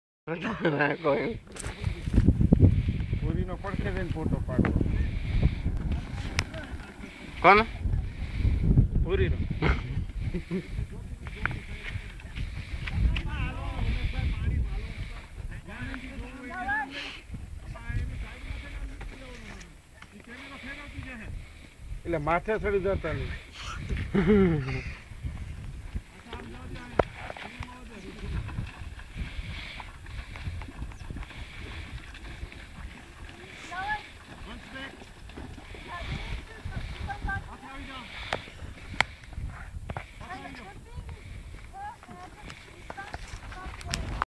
Chotila shoot
Equine Photographer KRISTEL RICHARDS doing photography of KATHIAWARI horse near Chotila ( Kalasar Talav) for the forth coming Book on KATHIAWARI Breed of Horses.